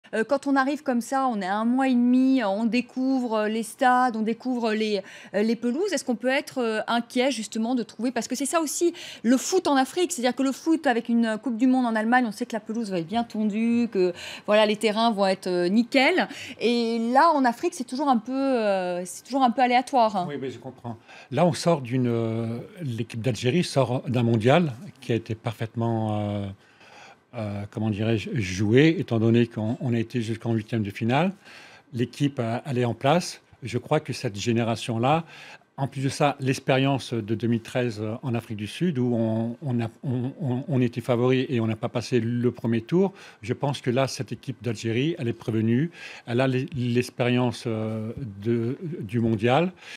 Nourreddine Kourichi : "L'équipe d'Algérie sort d'un mondial parfaitement réussi, elle est en place"
Nourreddine Kourichi, ancien sélectionneur adjoint de l'équipe nationale d'Algérie était le mercredi 3 décembre l'invité du DébatF24. Il revient sur le parcours de l'équipe nationale algérienne lors du Mondial 2014, plaçant les Fennecs parmi les favoris de la CAN 2015.